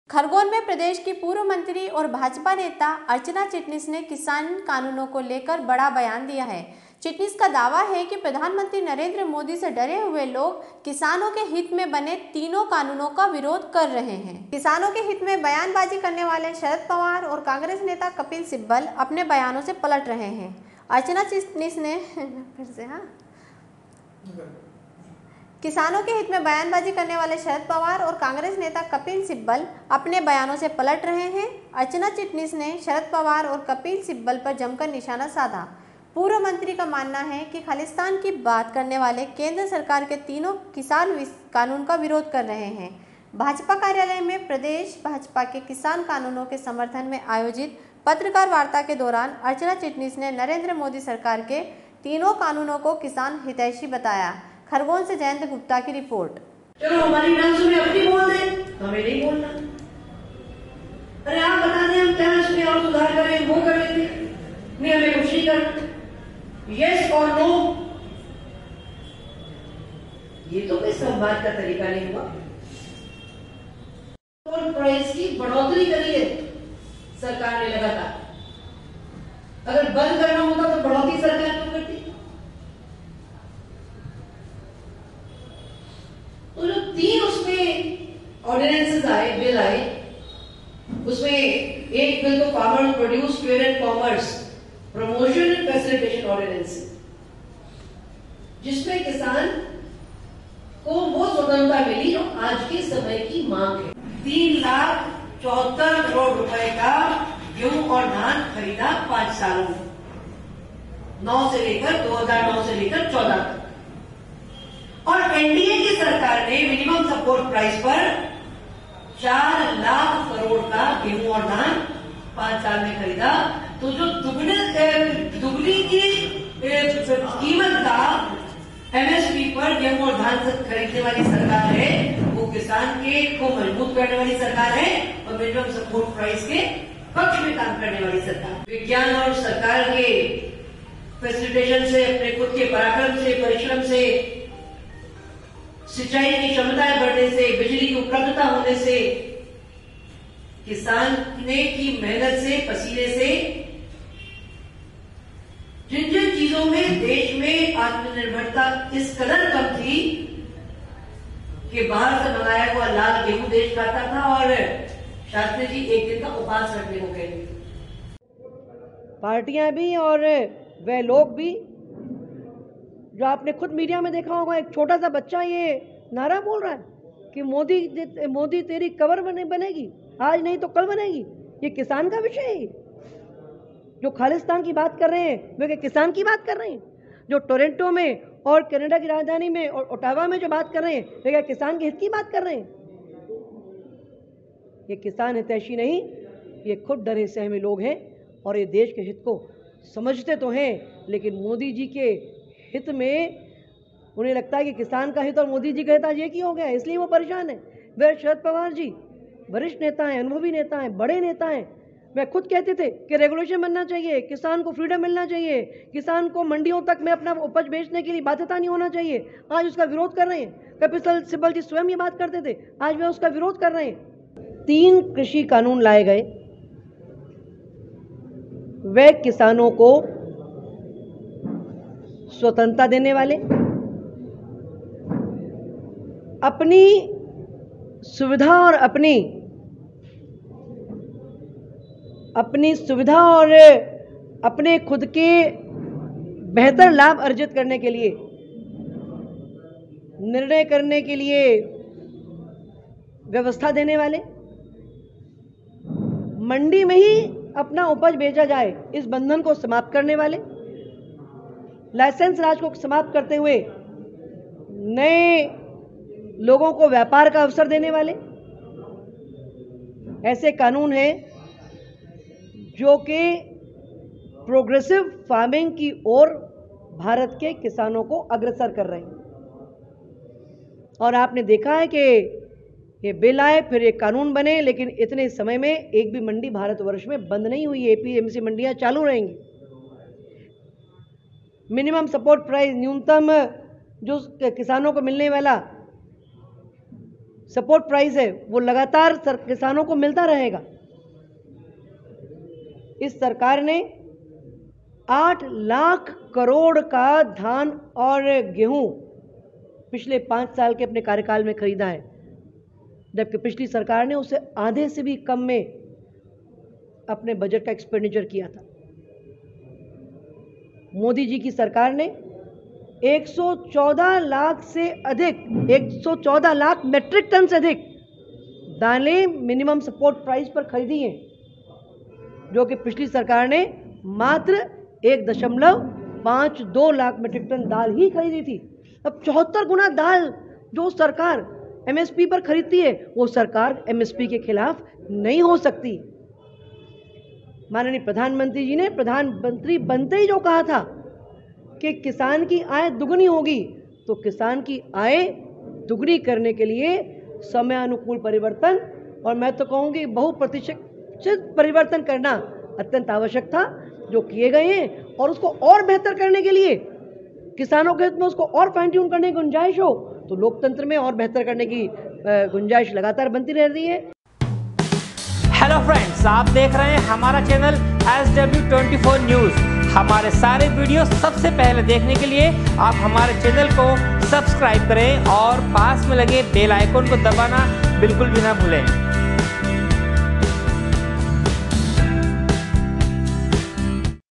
Khargone- प्रदेश की पूर्व मंत्री और भाजपा नेता अर्चना चिटनीस ने किसान कानूनों को लेकर बडाबयान दिया
Khargone- SW 24 News Madhyapradesh khargone SW24news #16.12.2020
खरगोन में प्रदेश की पूर्व मंत्री और भाजपा नेता अर्चना चिटनीस ने किसान कानूनों को लेकर बडा बयान दिया है। चिटनीस का दावा है कि प्रधानमंत्री नरेन्द्र मोदी से डरे हुए लोग किसानों के हित मे बने तीनांे कानूनो का विरोध कर रहे है।खरगोन से जयंत गुप्ता की रिपोर्ट।